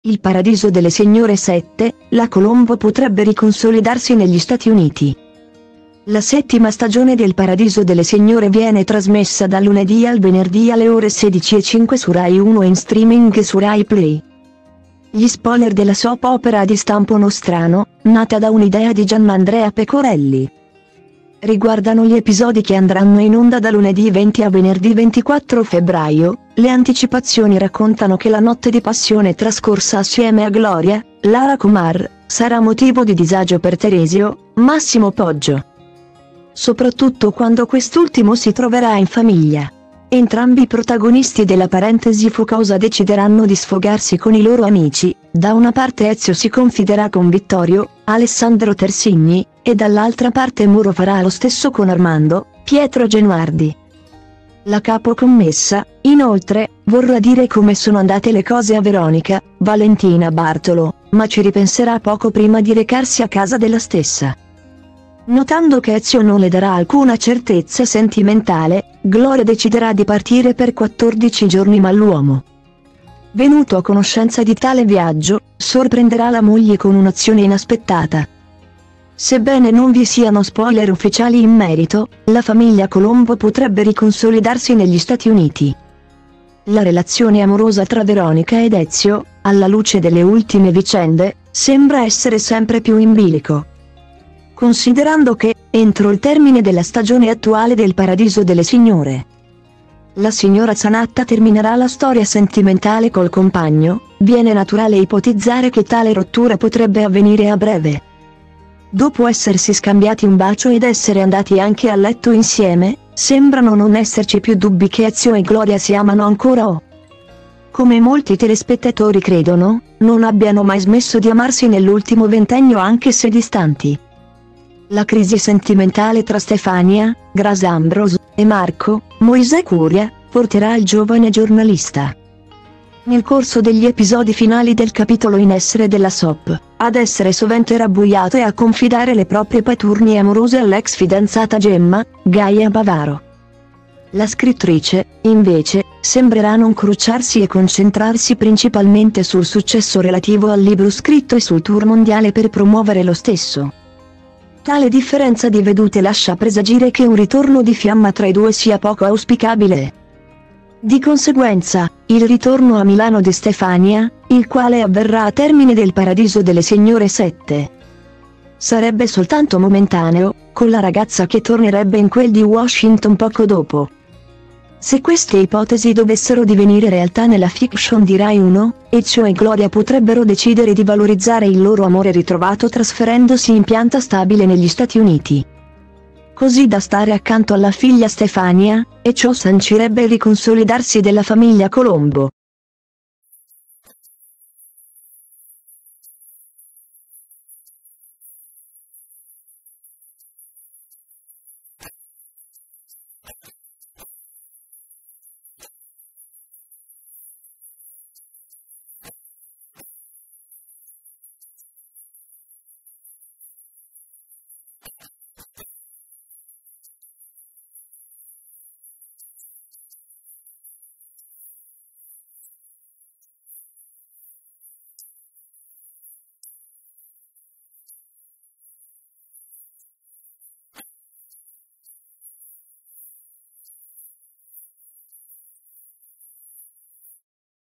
Il Paradiso delle Signore 7, la Colombo potrebbe riconsolidarsi negli Stati Uniti
La settima stagione de 'Il Paradiso delle Signore' viene trasmessa dal lunedì al venerdì alle ore 16:05 su Rai 1 e in streaming su RaiPlay.Gli spoiler della soap opera di stampo nostrano, nata da un'idea di Giannandrea Pecorelli,riguardano gli episodi che andranno in onda da lunedì 20 a venerdì 24 febbraio: le anticipazioni raccontano che la notte di passione trascorsa assieme a Gloria (Lara Komar) sarà motivo di disagio per Teresio (Massimo Poggio),